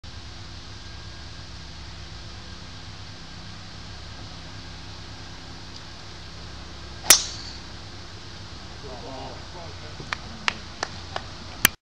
Jeffrey Kang begins his final round
U.S. Collegiate Championship
Final Round
Tuesday, October 25, 2011
The Golf Club of Georgia (Lakeside Course)
Alpharetta, GA